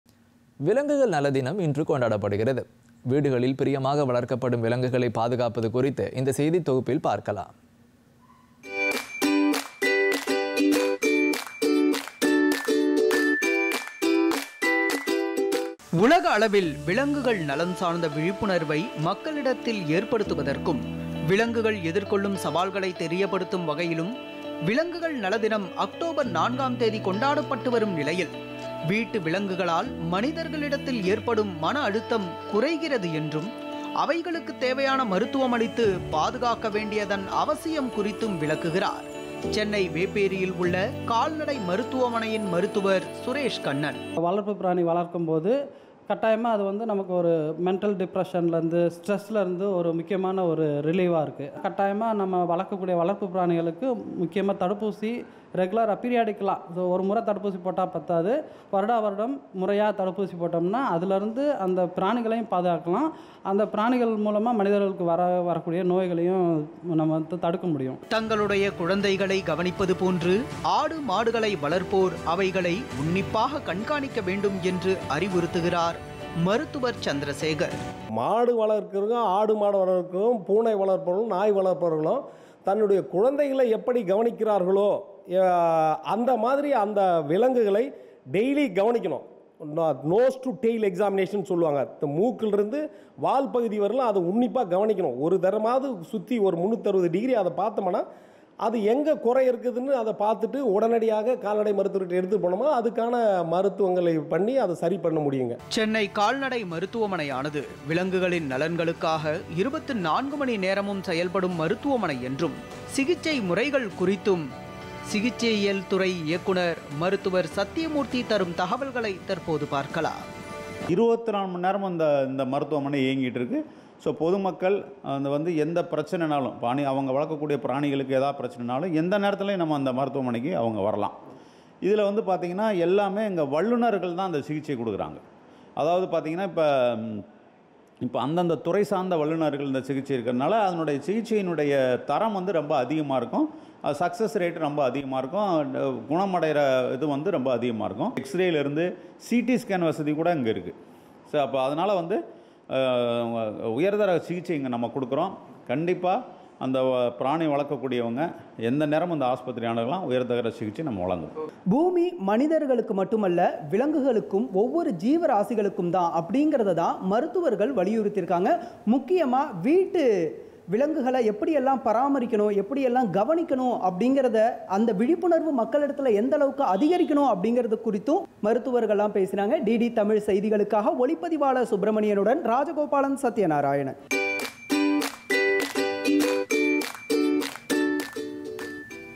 AnimalWelfareDay | விலங்குகள் நல தினம் இன்று
விலங்குகள் நல தினம் இன்று கொண்டாடப்படுகிறது. வீடுகளில் பிரியமாக வளர்க்கப்படும் விலங்குகளை பாதுகாப்பது குறித்து இந்த செய்தி தொகுப்பில் பார்க்கலாம்..